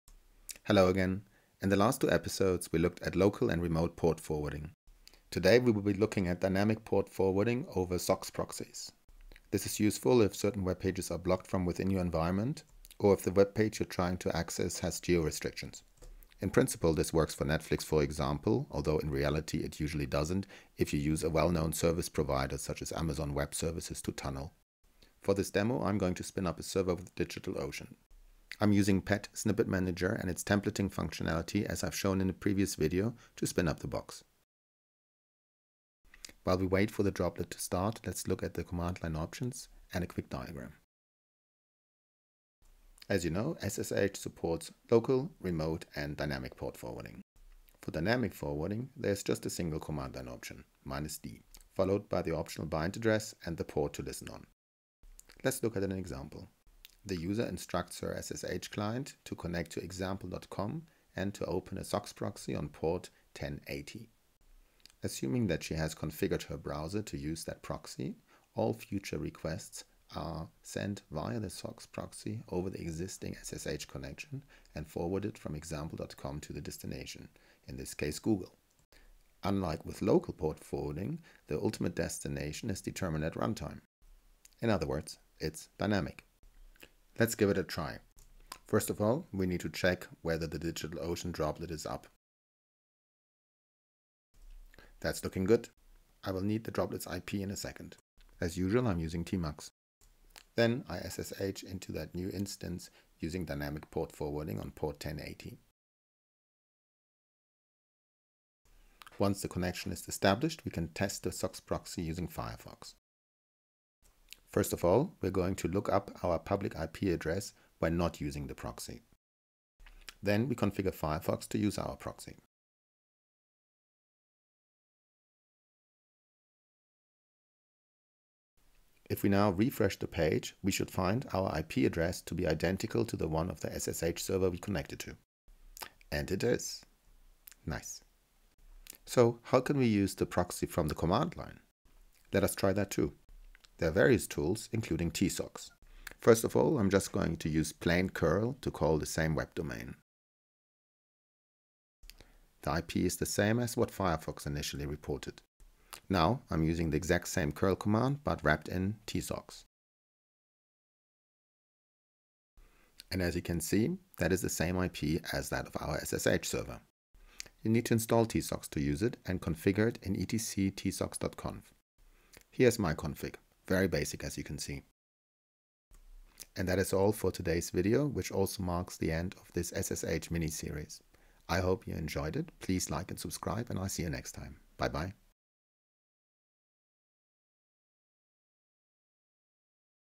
SSH tunnel for dynamic port forwarding
Learn how to create an SSH tunnel to dynamically forward request via a SOCKS proxy.

This is can be useful to bypass web filters or to circumvent geo restrictions by making your connection appear to originates from another location.